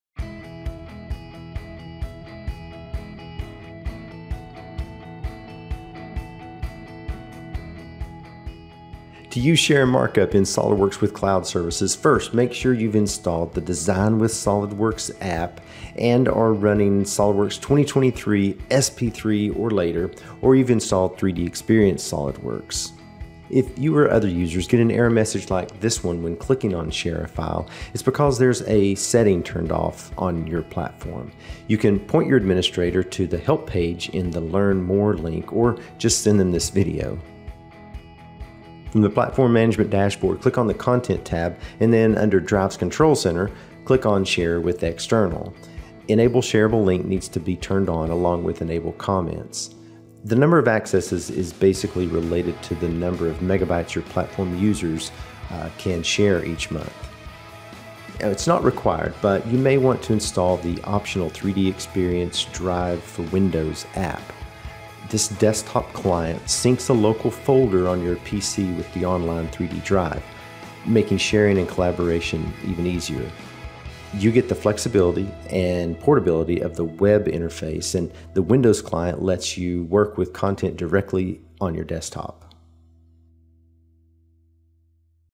Setup Required for Share and Markup
Learn the simple setup tasks you need to complete before sharing your designs in SOLIDWORKS.